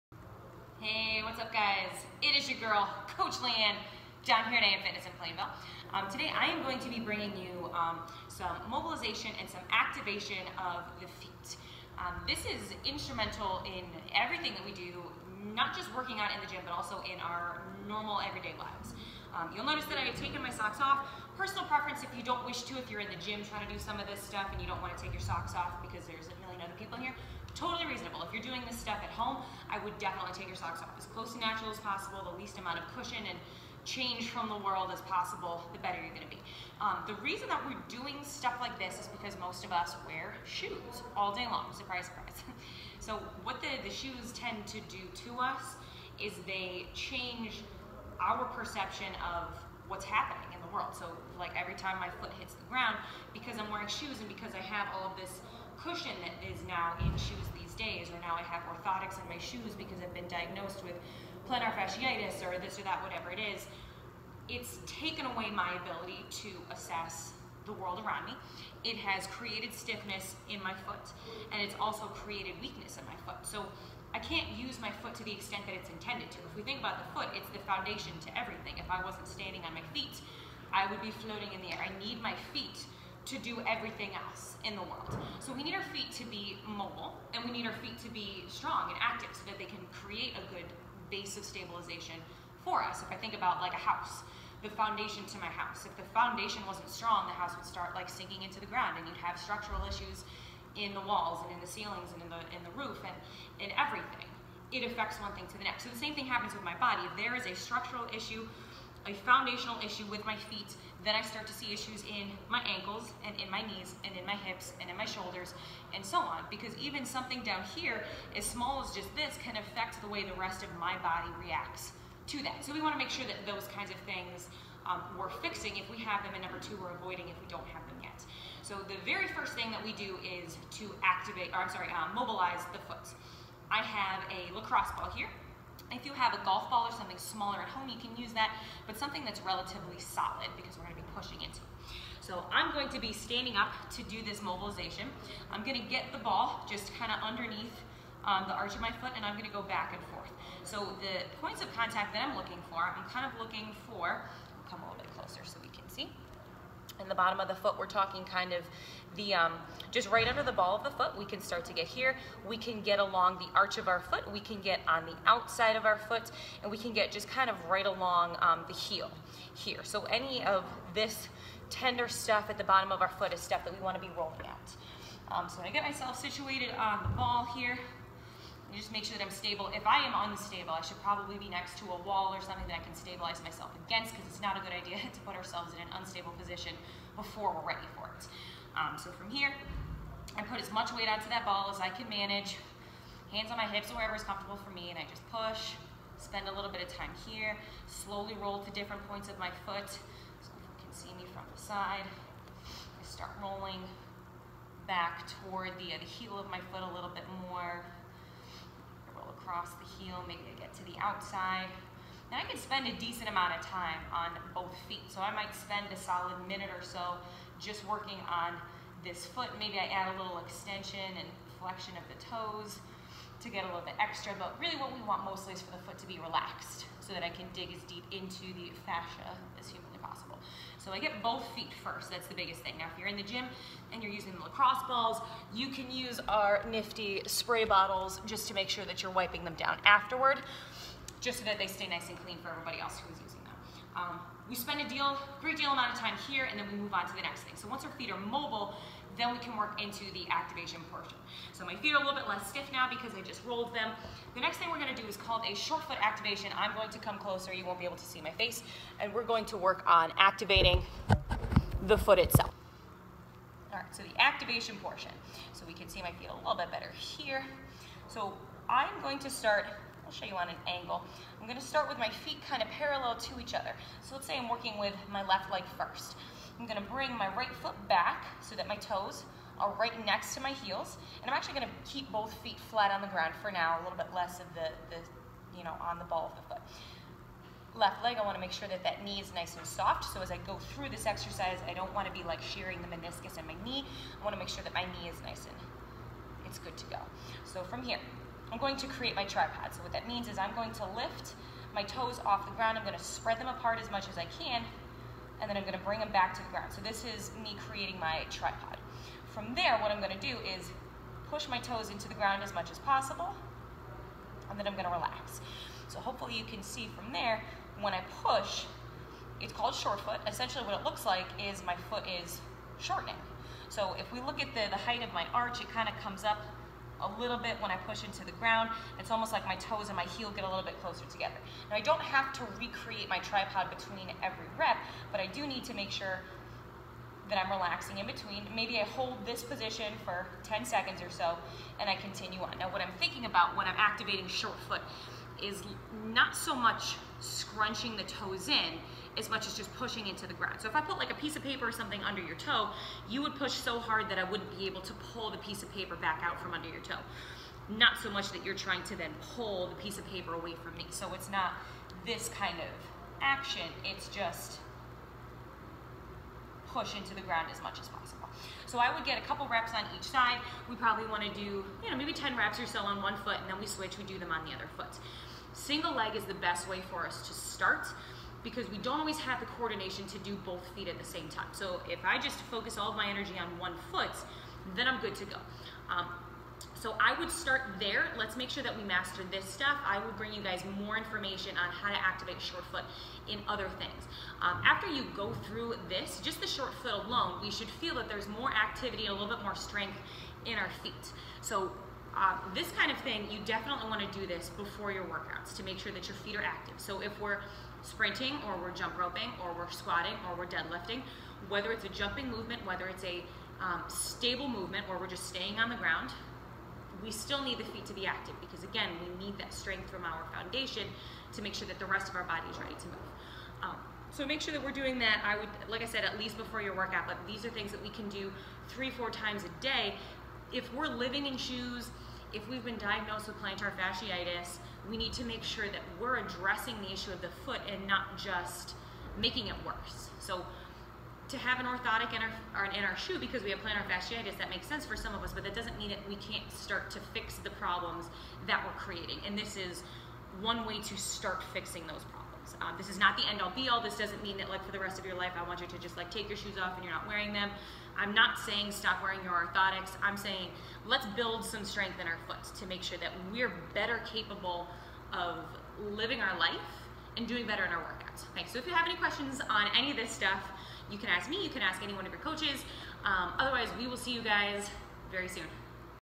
Short Foot Activation
We live in a society where shoes are accepted as a natural part of life. We weren’t born with shoes on our feet, we didn’t learn to walk by taking the floor away from our foot. And yet, we have learned that in order to walk properly, we must put a ton of cushioning between our foot and the ground, and we must force our foot into a locked position with insoles and orthotics. And in the process, we have lost our ability to interpret how hard our foot hits the ground, the ability to let the ankle joint move freely, and we have created stiff, weak feet that can no longer do what they were intended to do; support the rest of our body.

Now we have to relearn how to stabilize our body with our foot and not just our big muscles like the quads and glutes.

Picture the foundation of a house. If the foundation is compromised, the rest of the structure becomes compromised; the walls break, the ceilings fall, the roof collapses, the windows break. If your foot is your foundation, you don’t want it to be broken. If it is, then the rest of your body's structure will break too. If you experience discomfort, hypomobility, injury, etc, at the ankle, knee, hip, shoulder, or neck, your foot may very well be to blame. Because once one structure breaks, the rest will follow suit.

But how do I repair the structure? Whether you have plantar fasciitis, IT Band syndrome, shin splints, most of these problems can be aided and even fixed by mobilizing and activating the foot. This can be done in many ways, but the foundation of movement is the same. Watch my video to find out how to mobilize and activate your foot so the healing process can begin!